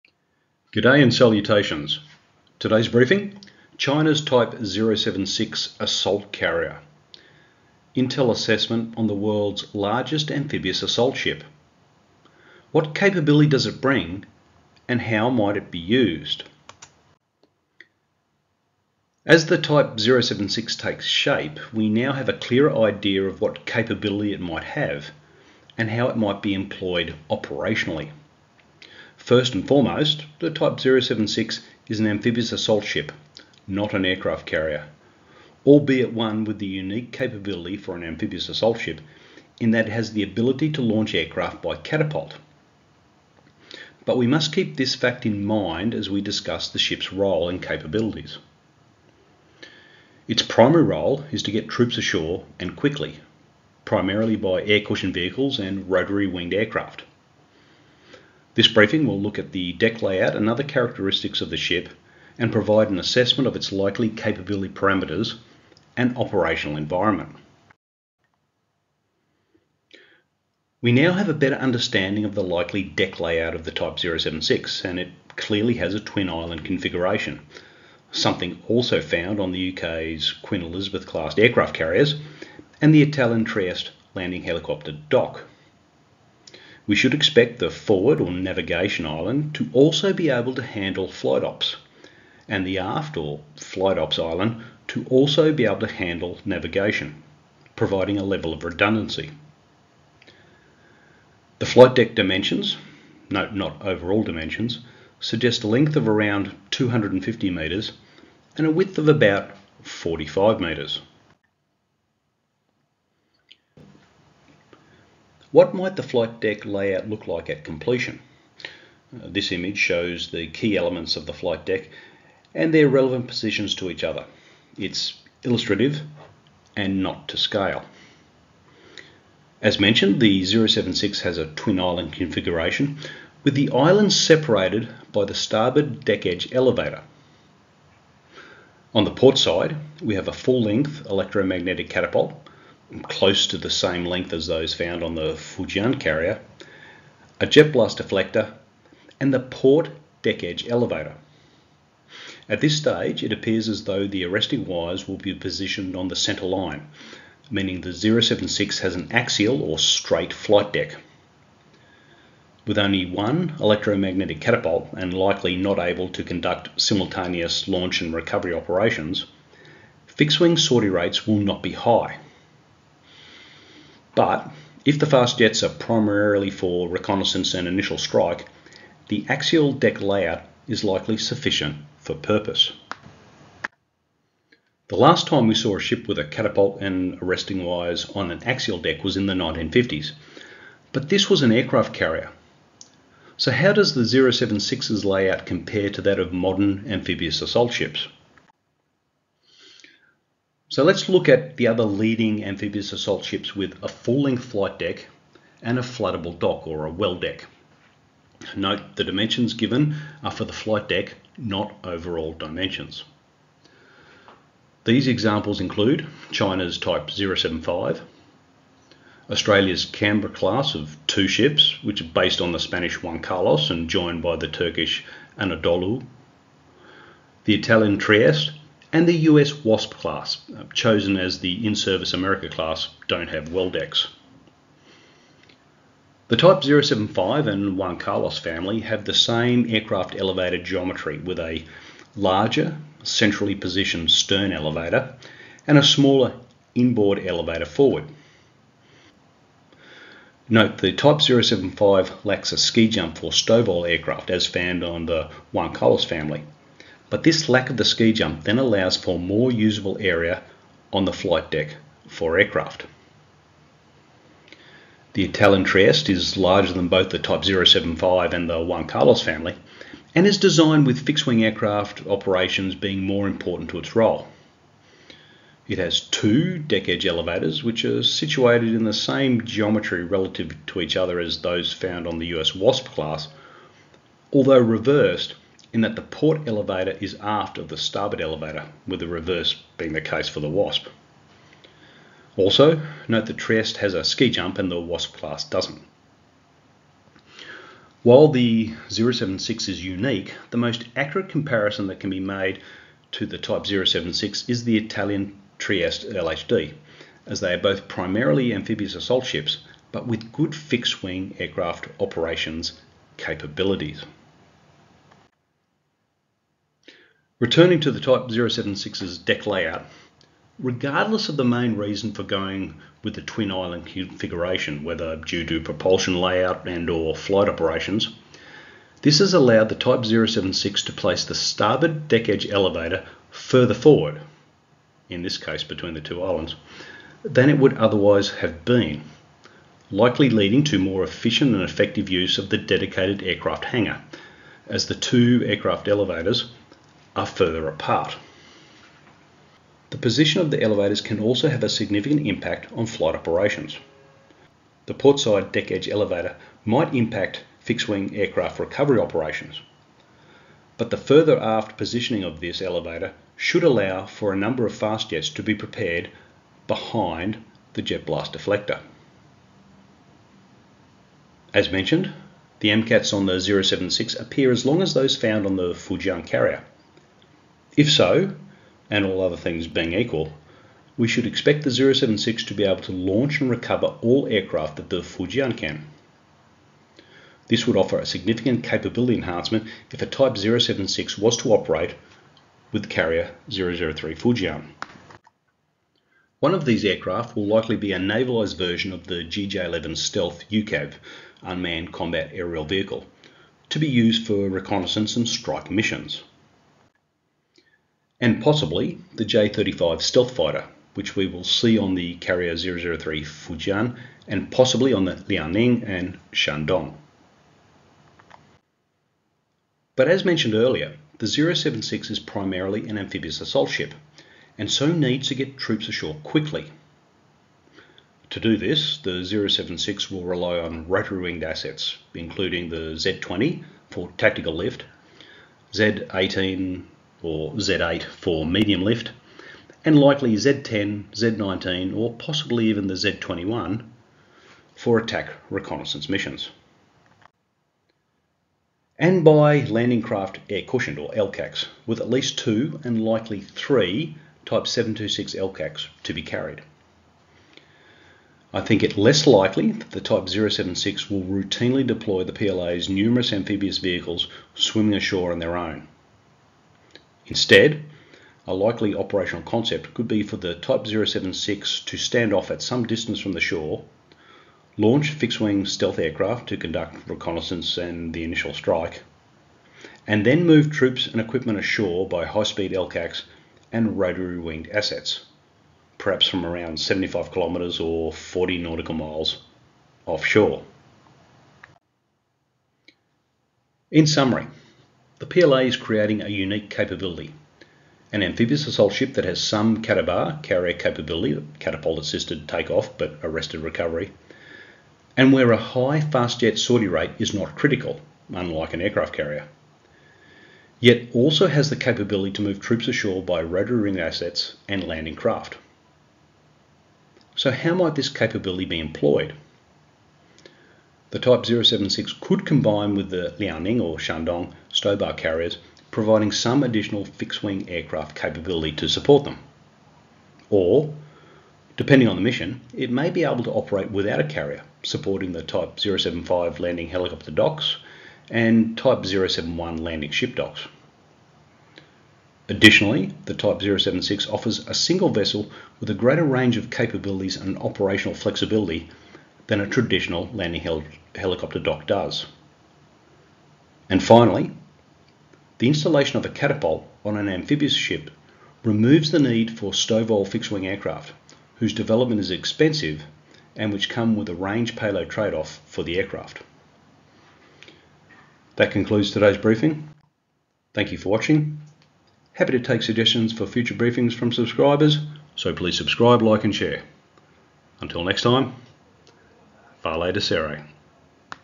China's Type 076 Assault Carrier - A unique capability
Intel Assessment on the world’s largest amphibious assault ship – what capability does it bring, & how might it be used?

Sources: PLA website
Italian Navy
U.S. Department of Defense